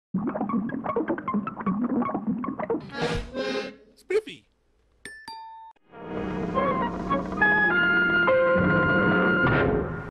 Freshing Equalizer Spiffy Pictures
Reupload for Megan Woodmansee because he deleted all his logo editing videos!!! For some reason he has changed his channel name and he seems to have lost all of his subscribers. (I was one of them) He now only has 12 subscribers. (I remember he had 18.4k subscribers before I think) So, I don't know what the hell happened to his channel. :(
He said he will not bring back his logo editing videos as stated in his channel description so I am bringing them back myself.